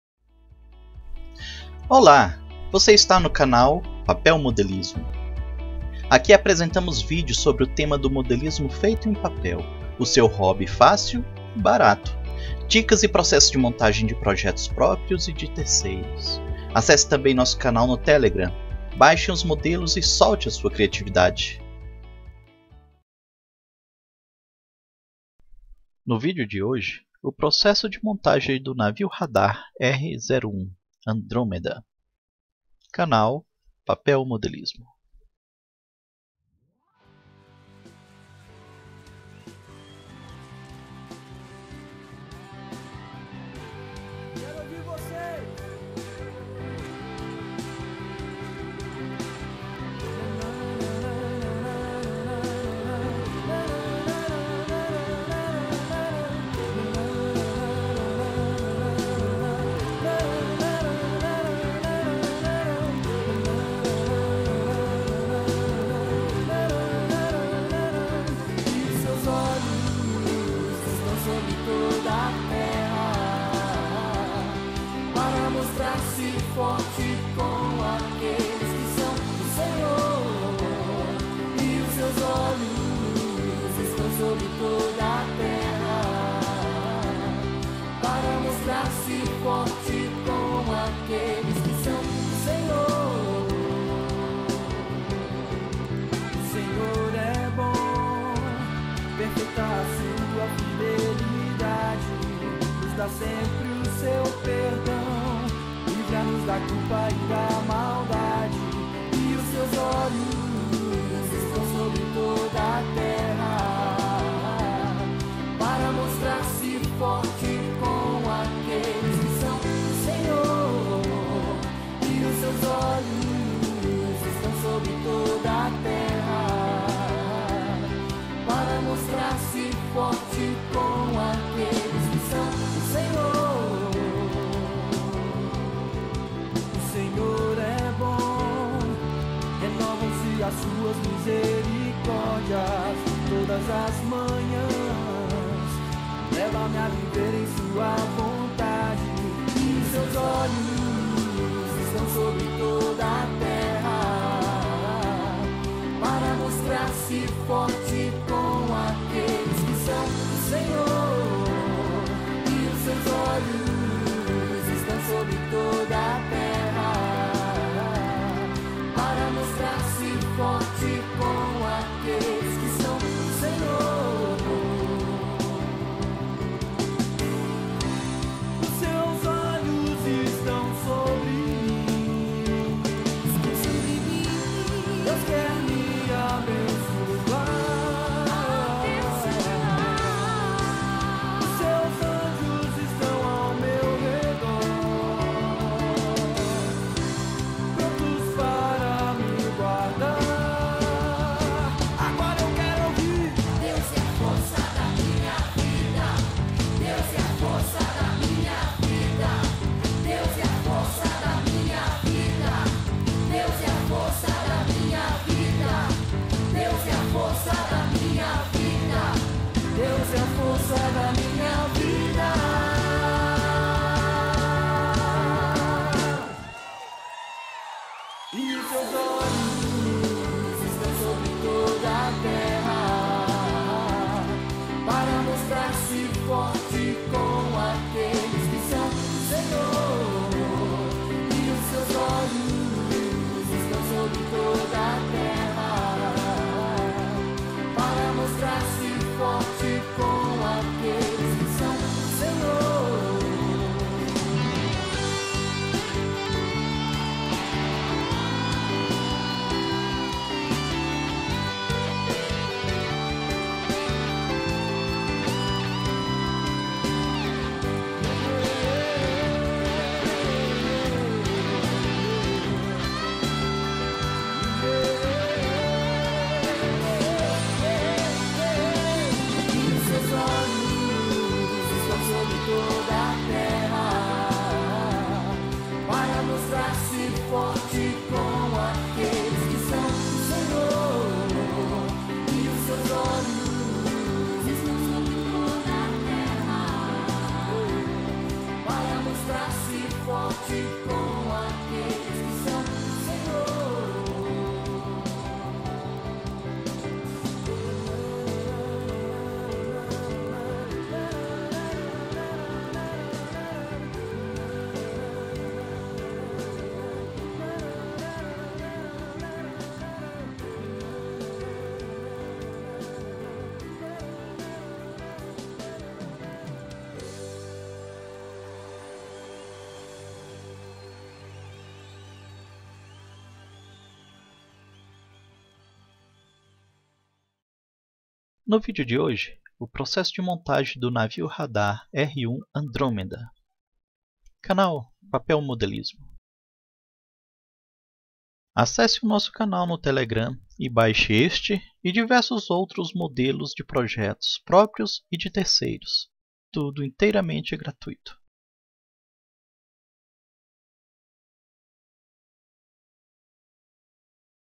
Vídeo 07 - R01 ANDRÔMEDA - PAPELMODELISMO
PROCESSO DE MOSNTAGEM DO NAVIO RADAR - R01 ANDRÔMEDA
Projeto Próprio - Canal Papel Modelismo

PAPELMODELISMO